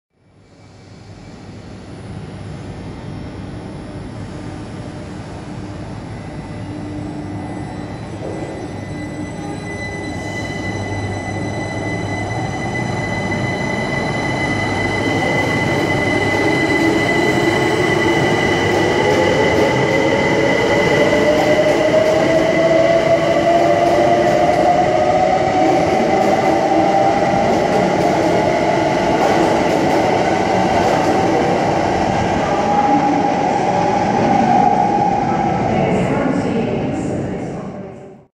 Class 387 124 & 127 at St. Pancras Thameslink
After arriving at St. Pancras Thameslink for the 1st time in my life, on my 1st ever Class 319, what happens to pull in? Only TWO Class 387s bound for Bedford! (I had to run to the end of the platform to catch this, be grateful) :P